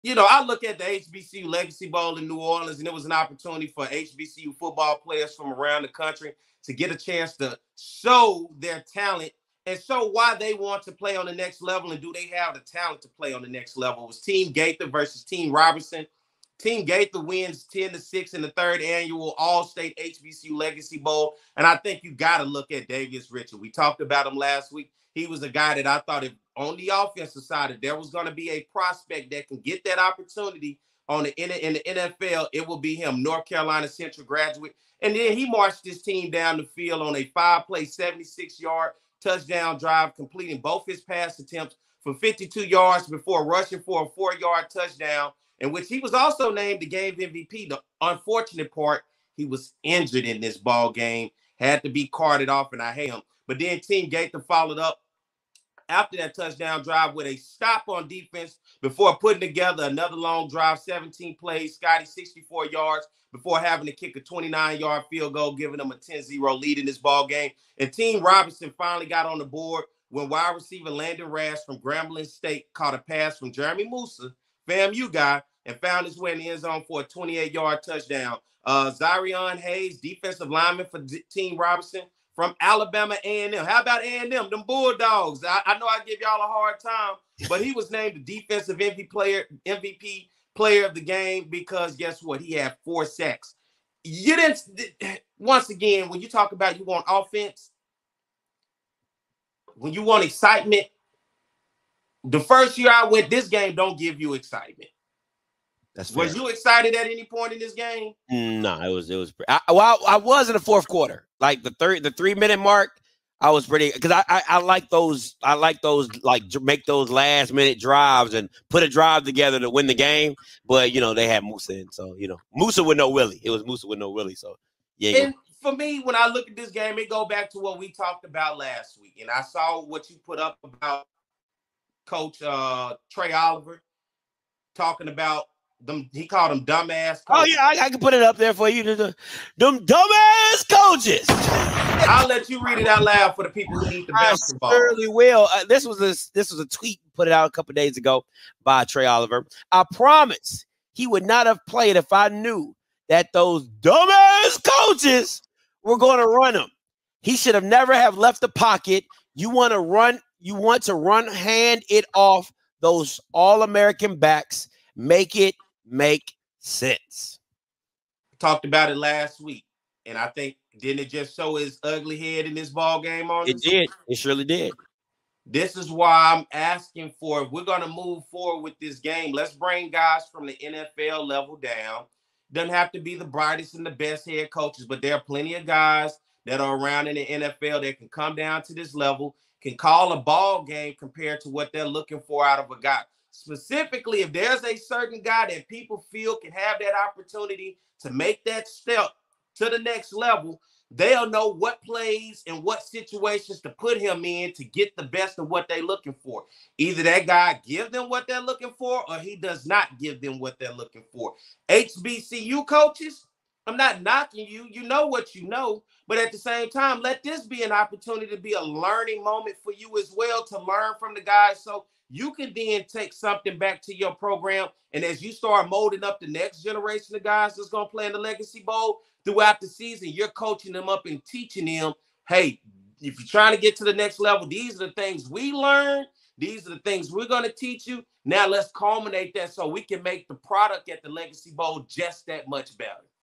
2024 HBCU Legacy Bowl Recap
2024 HBCU Legacy Bowl Recap! Perry White & Scotty break down the game as well as talk about comments made from NCCU’s Head Football Coach Trei Oliver